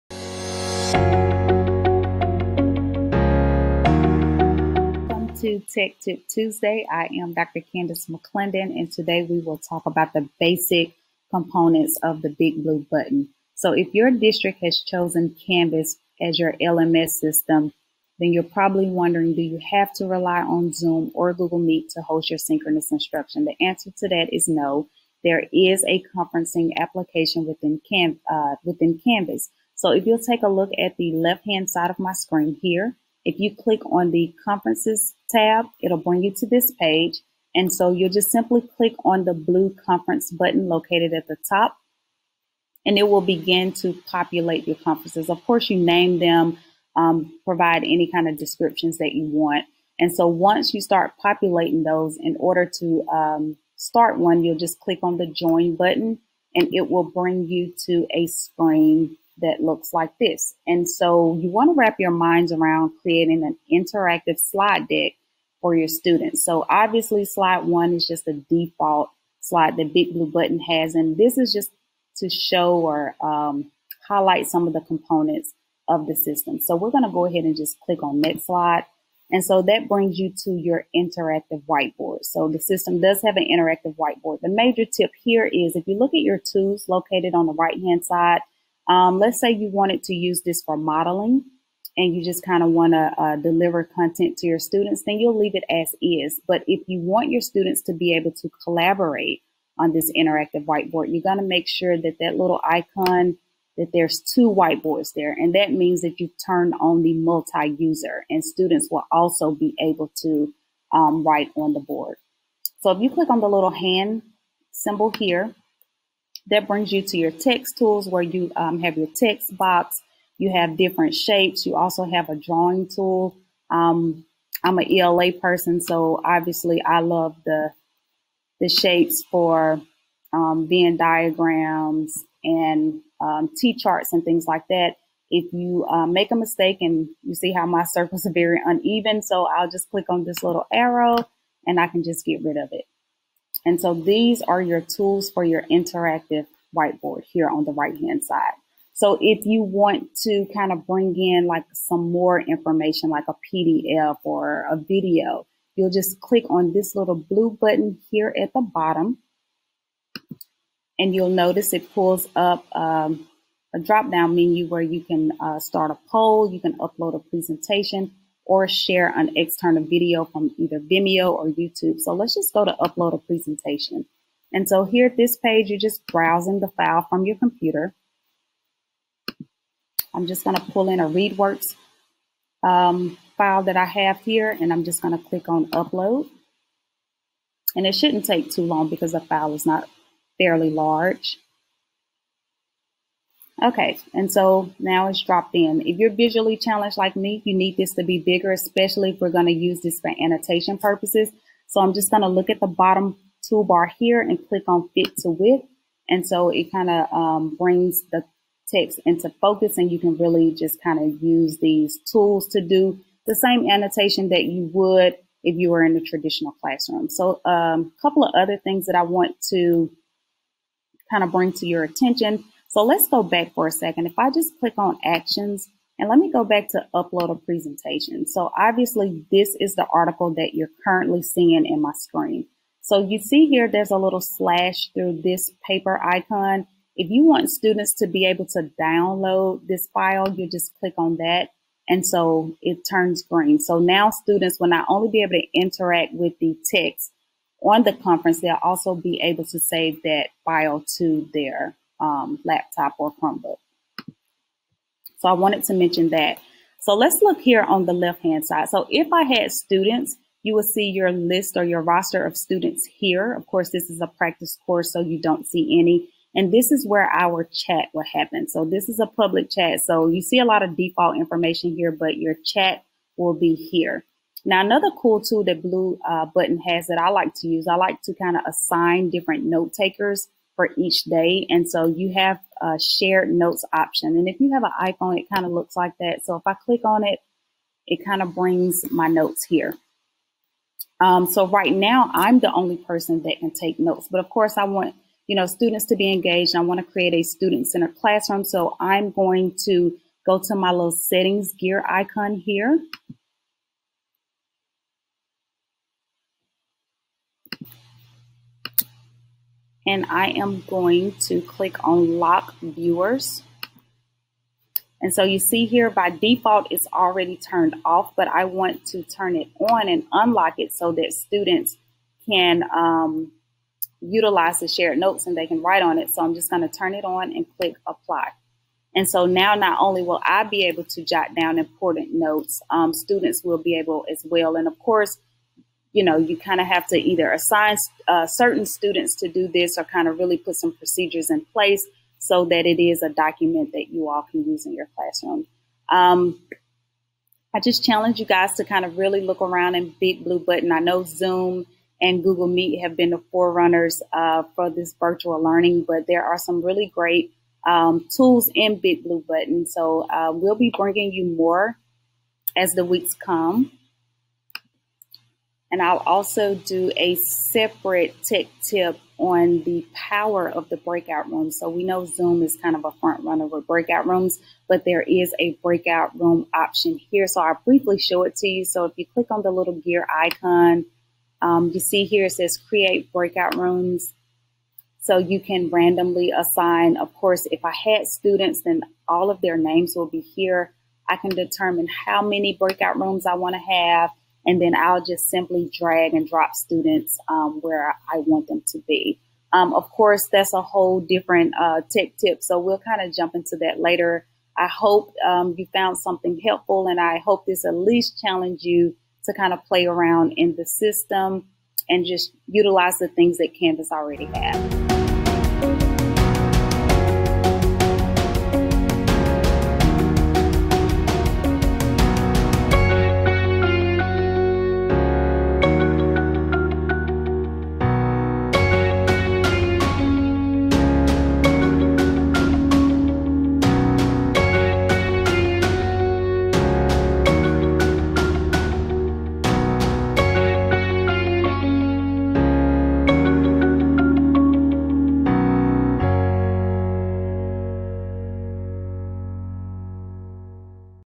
Big Blue Button in Canvas
In today's tech tip, ELA Instructional Coach Dr. Candace McClendon demonstrates how to use the Big Blue Button in Canvas.